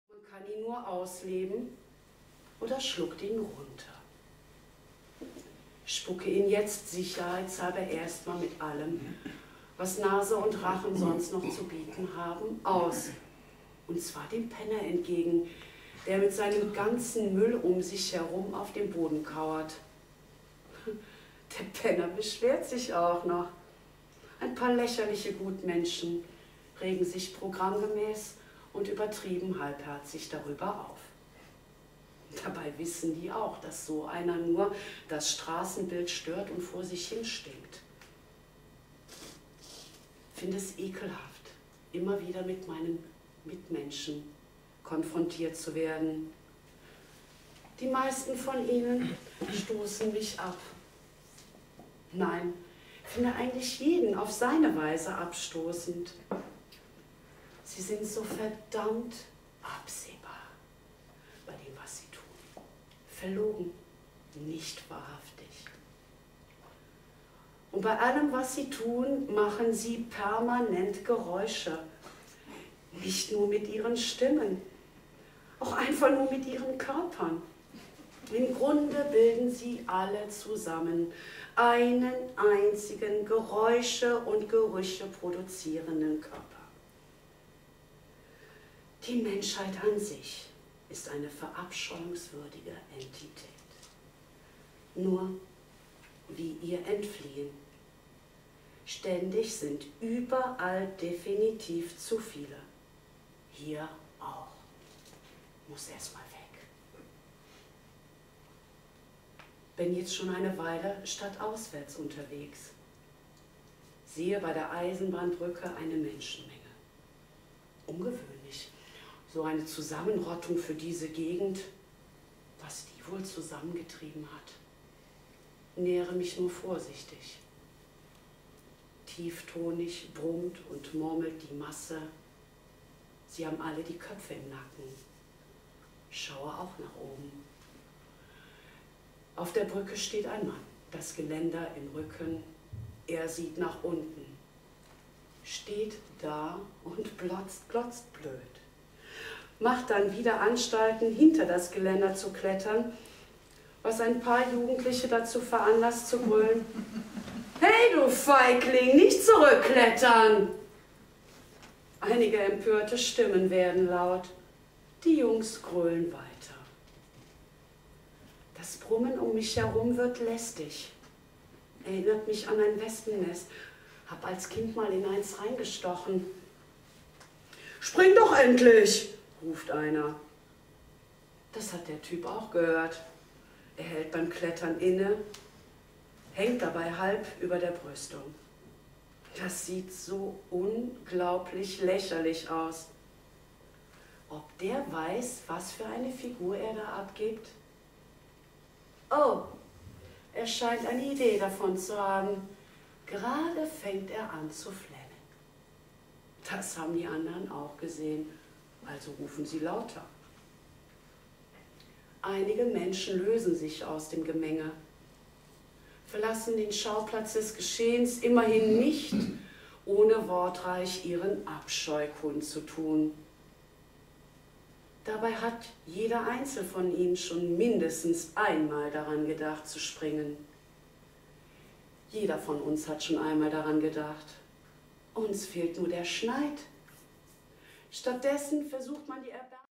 Elly Lindemann - "Der Erlöser"
Genre: komisch im Sinne von seltsam. Ausschnitt aus der 15. Lesung des 'Kalker Kaffee'.